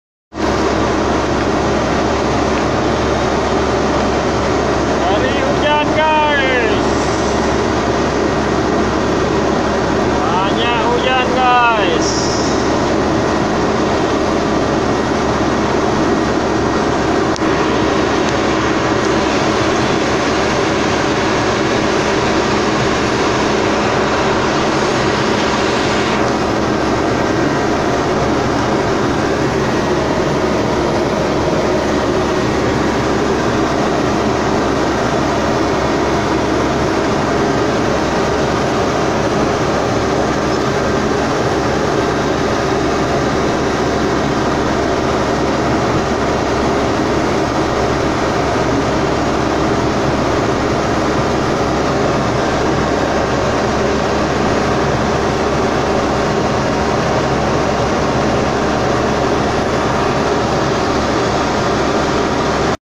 Excavator pun pandai berenang
Berenang sambil membawa kayu. Hebat tak Aku sebagai excavator